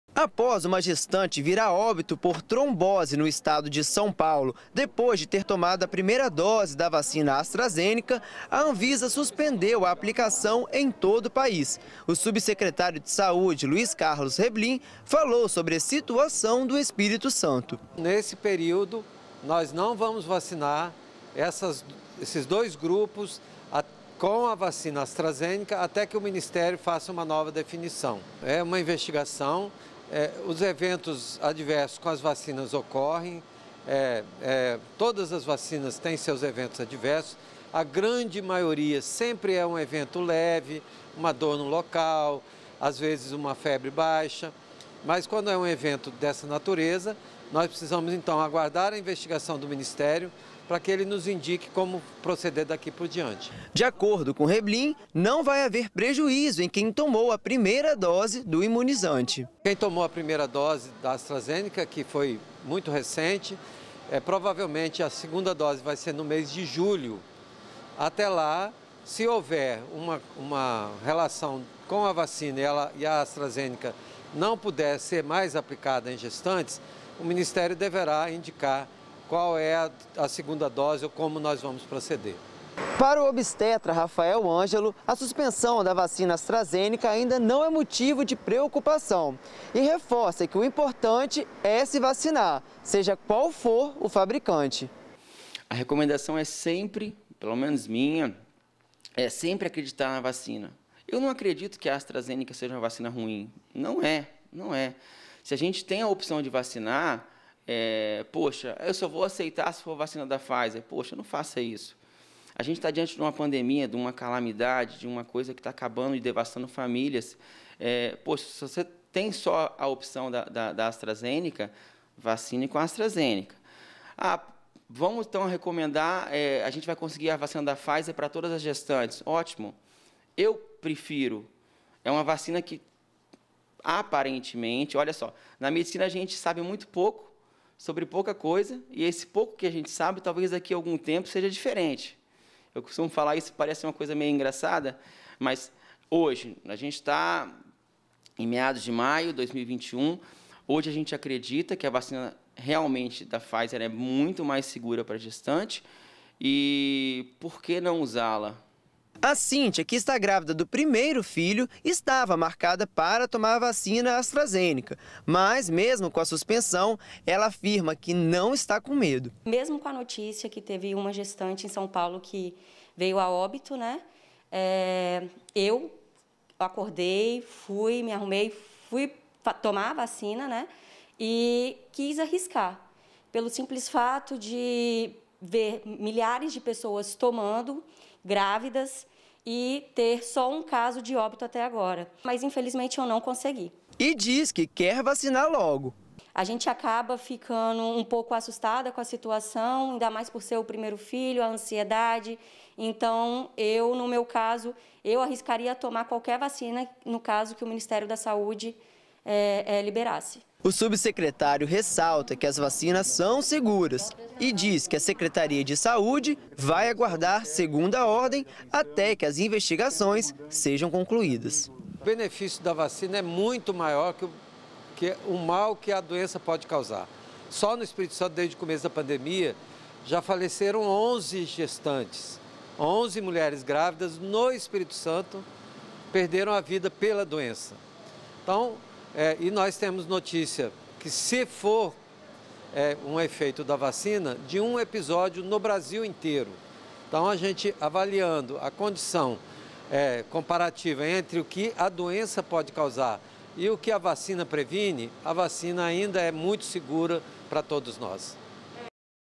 Anvisa suspende a aplicação da vacina Astrazeneca em grávidas
A Agência Nacional de Vigilância Sanitária, a Anvisa, recomendou suspender a aplicação da vacina AstraZeneca em mulheres grávidas. A orientação é que gestantes tomem apenas as vacinas da Pfizer ou da CoronaVac.